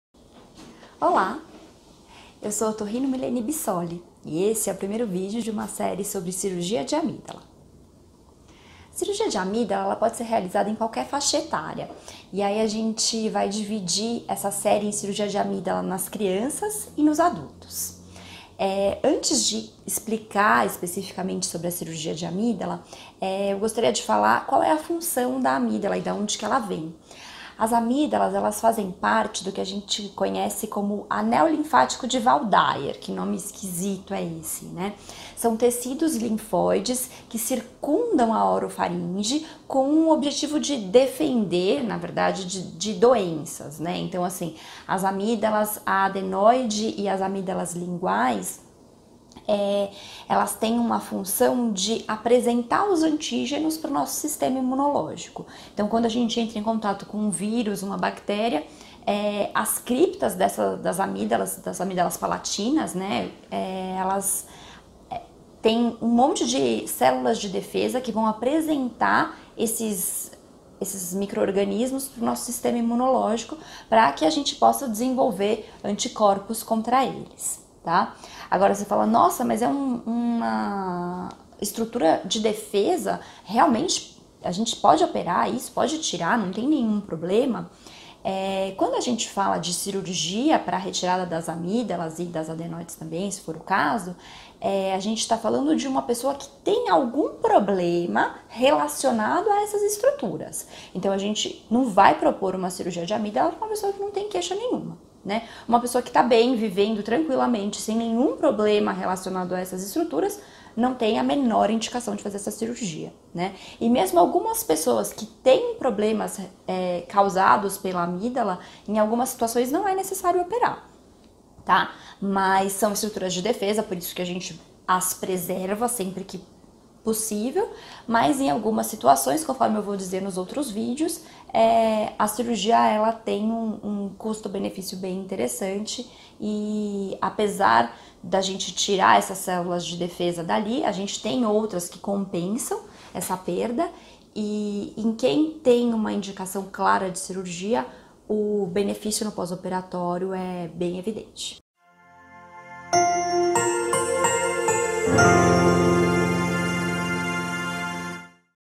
Cirurgia de Amígdala: função e anatomia das amígdalas
Amígdalas fazem parte do tecido linfoide de defesa da Faringe, são órgãos apresentadores de antígenos para o sistema imune.

Apesar de serem estruturas de defesa, sua remoção cirúrgica em pacientes que apresentam doenças nas amígdalas não causa problemas imunológicos no futuro, pois outras estruturas de defesa são preservadas e continuam funcionando normalmente.

As pessoas com doenças nas amígdalas que optam pelo tratamento cirúrgico têm ganho evidente na qualidade de vida no pós-operatório.